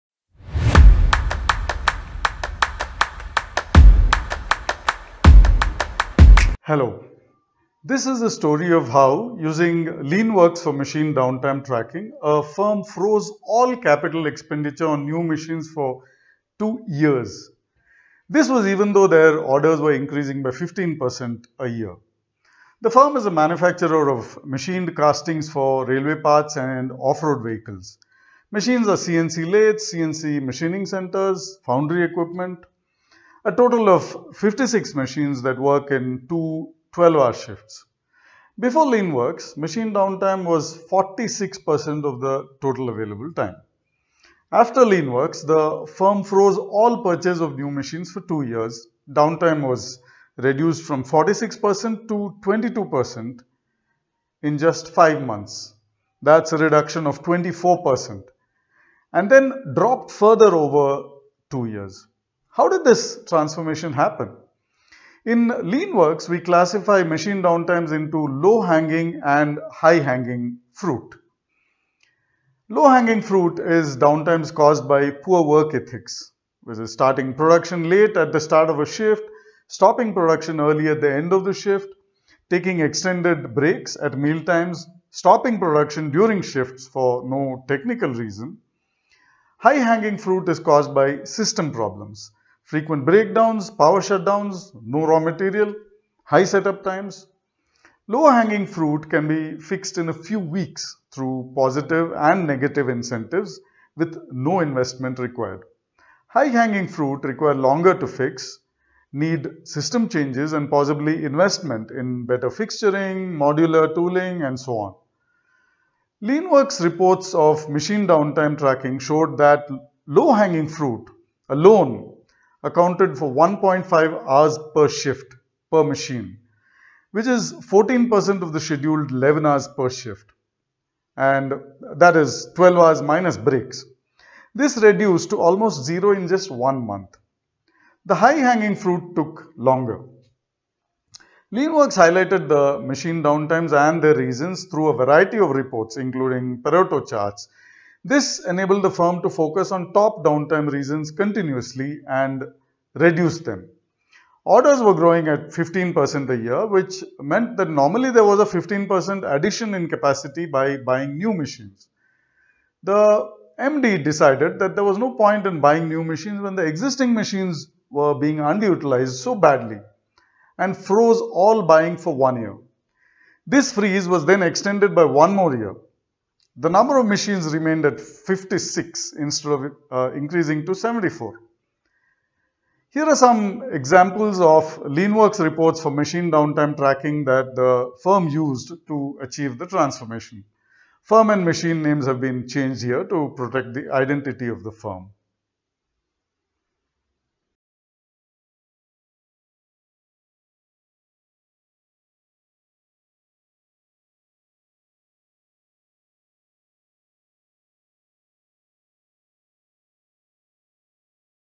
Machine downtime tracking case study
CAPEX reduction - no new machines purchased for 2 years.

How to monitor manually operated machines with an Industry 4.0 system ?
What is the implementation cost ? Can I afford it ? How much time will it take ? What kind of manual machines is machine monitoring possible for?